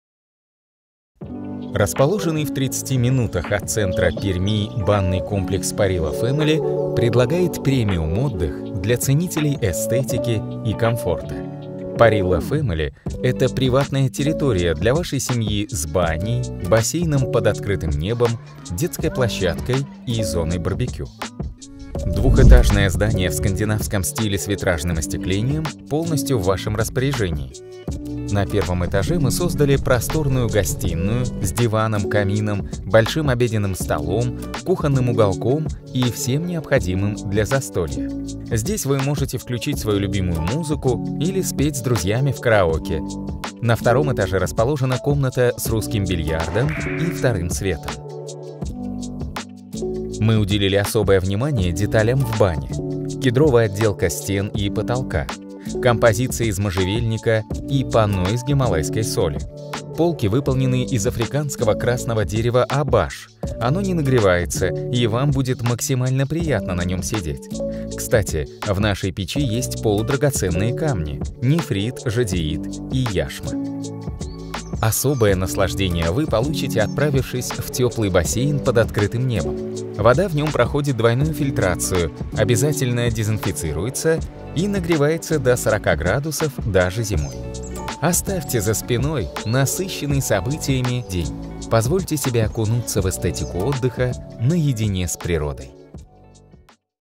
Видеообзор банного комплекса
Начинаем с обзора площадки  ( коптер) как будто приземляется на площадку, далее показываем все, что есть снаружи:
- мангальная зона (общий вид) + мангал, шампура (детали)
- бассейн (общий вид) +  лестница, подсветка (детали)
- Зона отдыха ( стулья снаружи) (общий вид_детали)
- Детская площадка (общий и детали)

Залетаем внутрь
- Зона гостинной общий вид и детали: кухня, инвентарь, обеденный стол, камин, тв,
- Сауна (общий вид) + дерево, сильвинит, панно на потолке, котел камни, масла (детали)
-Душевая (общий вид) + лейка, зеркало, плитка (детали)

Облет вверх по лестнице + слайд по перилам
- Бильярдная (общий вид) + шары, кий, полотно (детали)
- Пролет во второй свет + люстра